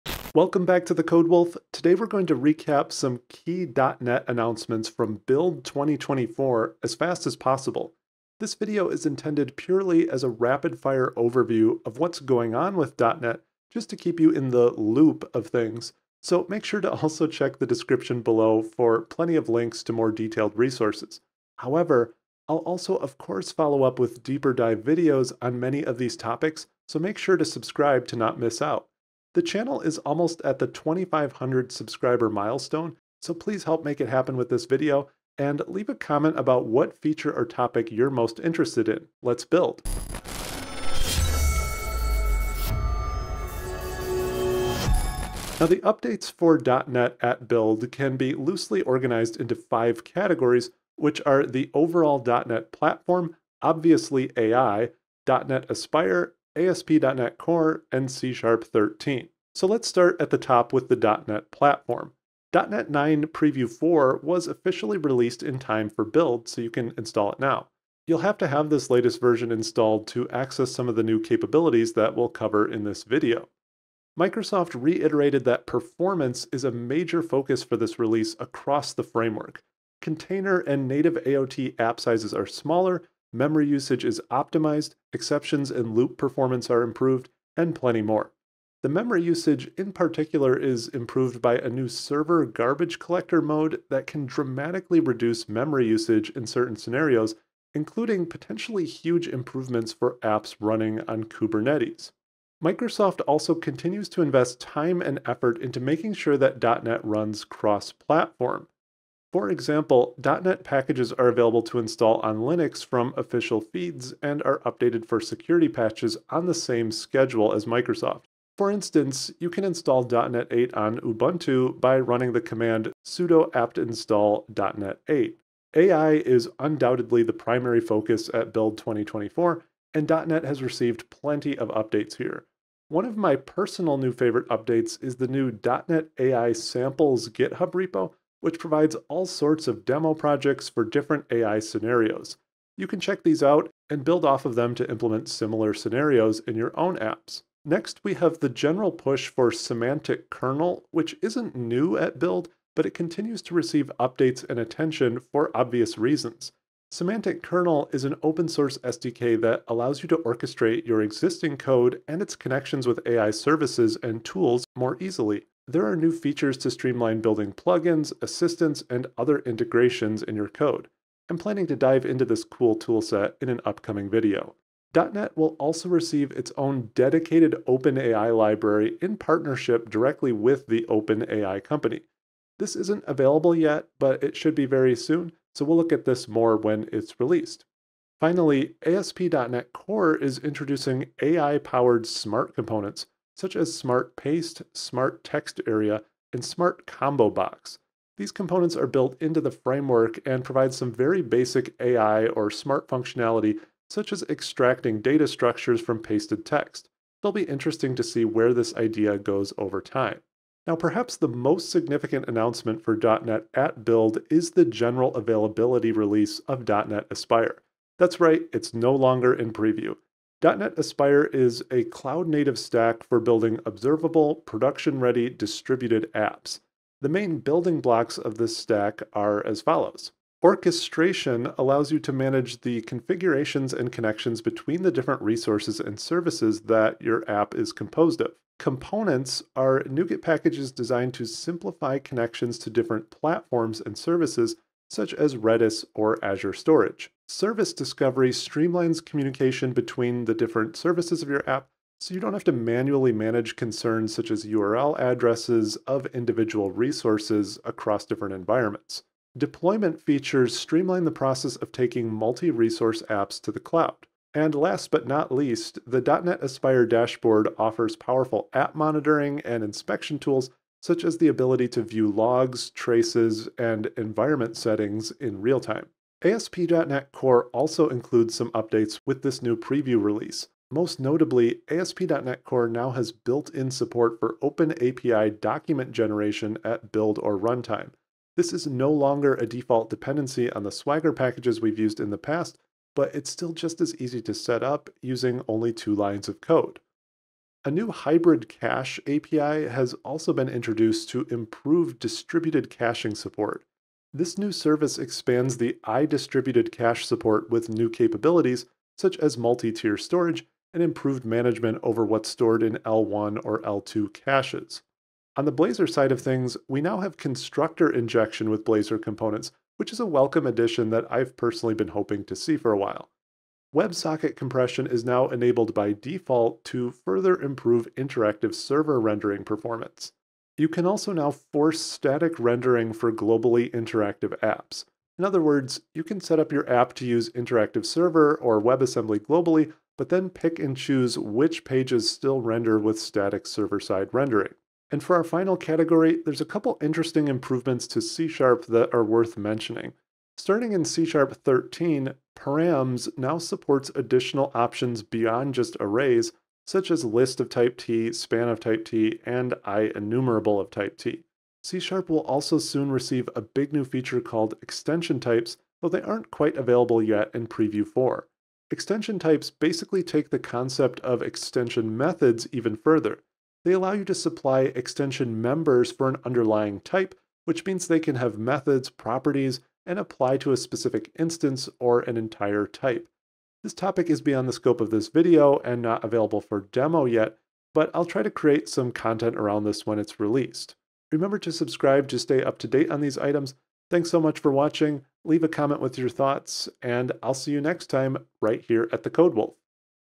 Microsoft Build 2024 - .NET Recap in 6 minutes!
There's an overwhelming amount of information at Microsoft Build 2024 - everything from AI updates to the .NET Aspire release to new platform features. In this video, we'll quickly explore the key announcements you need to know about for .NET 8.0 and 9.0.  This is not a deep dive into any single feature, but rather an overview of everything you should be aware of to stay in the loop and dive into later.